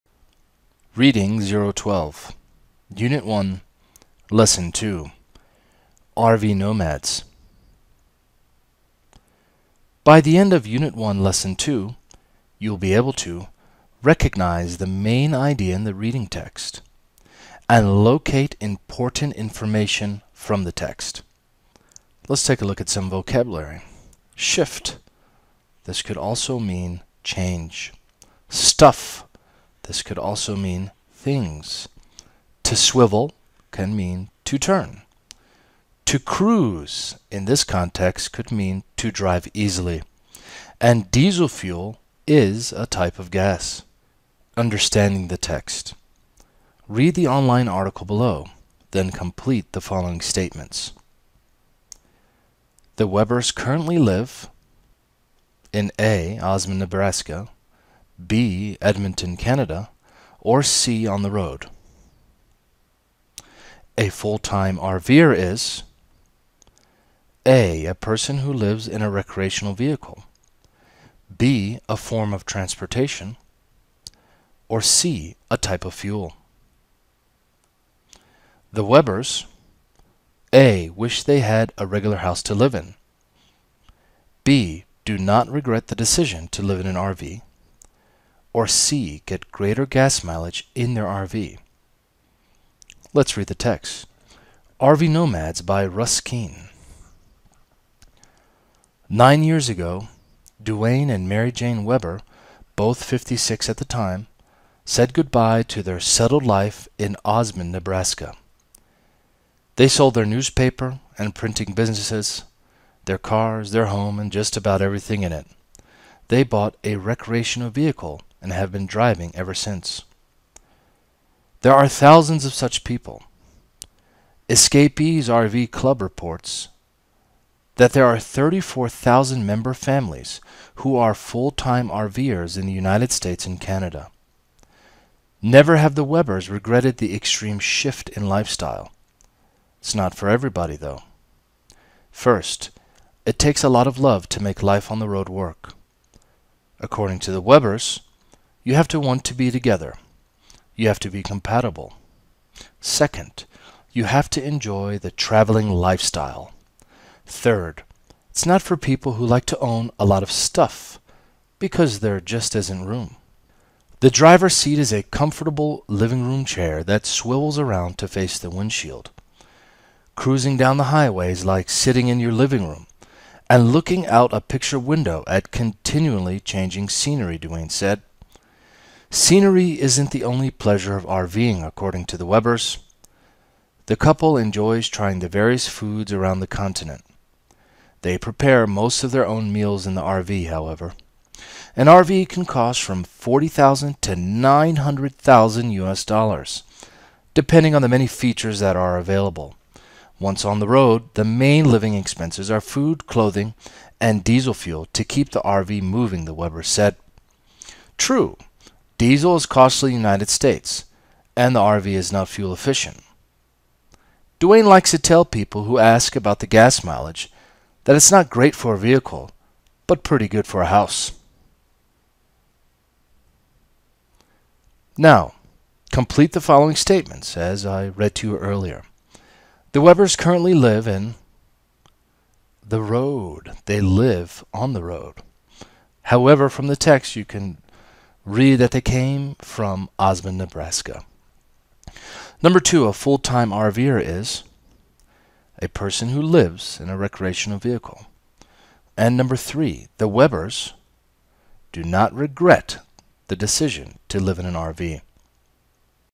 Intensive English | Reading U1-L2 | R V Nomads
المقررات المفتوحة - مقرر Intensive English
Reading  U1-L2 R V Nomads
 Recognize the main idea in the reading text
Locate important information from the text.